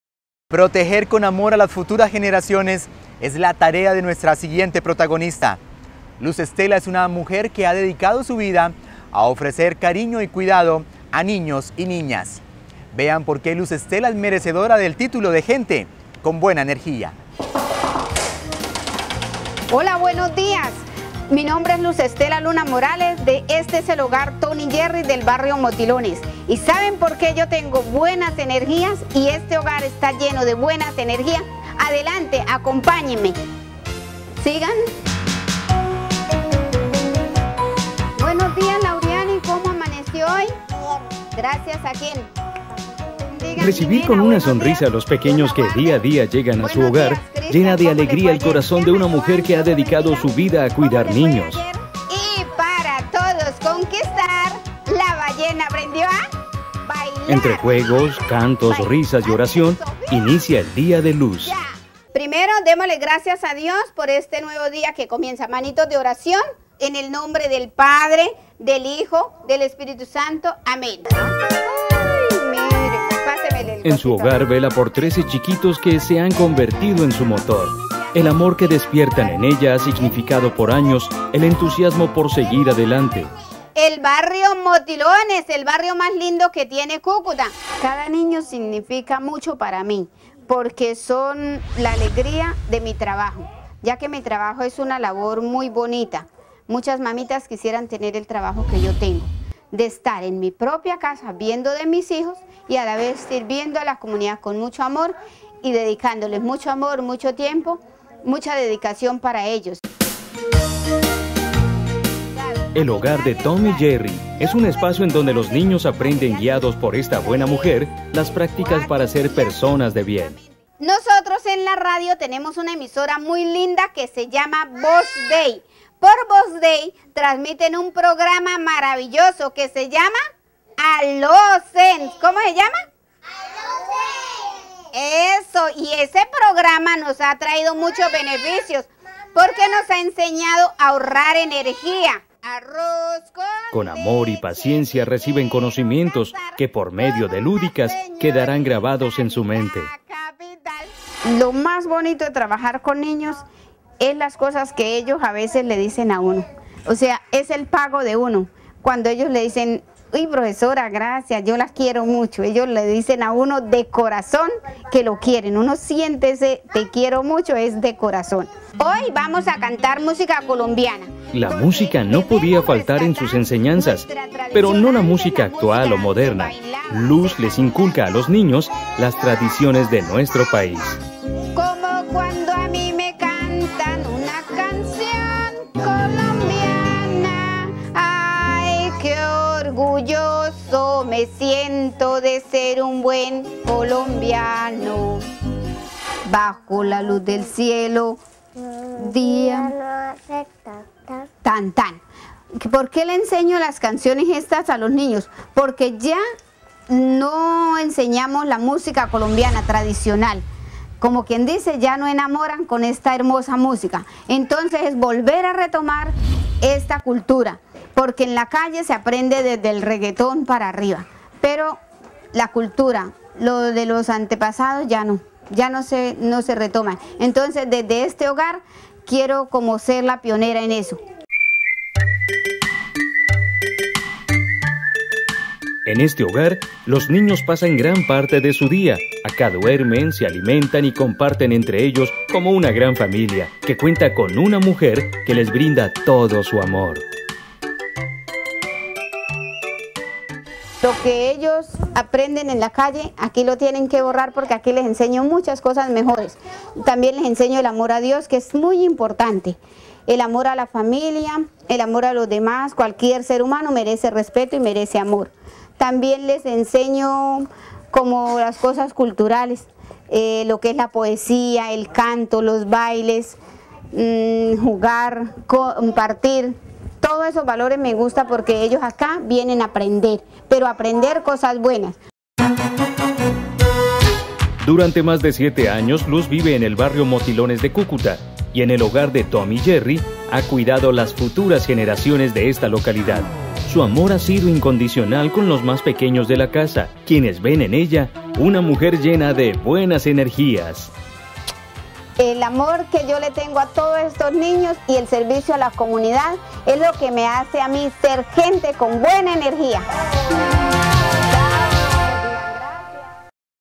Luz Stella Luna, Gente con Buena Energia
Con un corazón gigante, Luz Stella Luna vive su vida para los niños y niñas del barrio Motilones en Cúcuta.